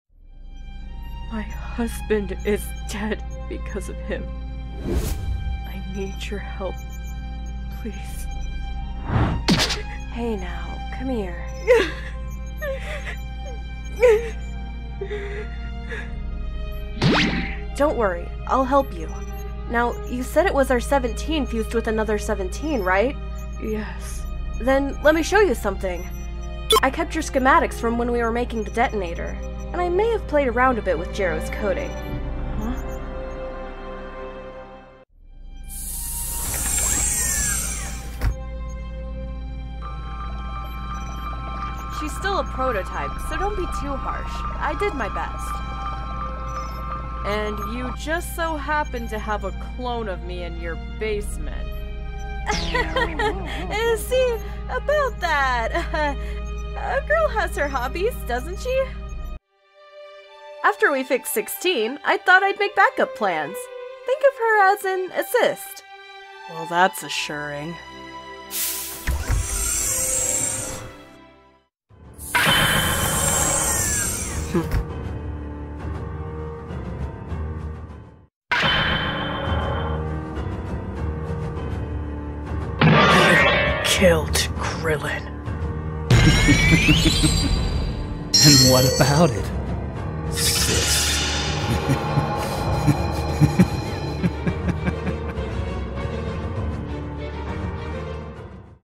Super 18 [Complete] Plus Extra | Dragon Ball GT Comic Dub
💥Dragon Cast💥

Voice Of 18 and Bulma: (mbteddygram)

Voice Of Super 17 @DandelionandBardock

Have you ever seen Super 18 | Dragon Ball GT Comic Dub before watching this video?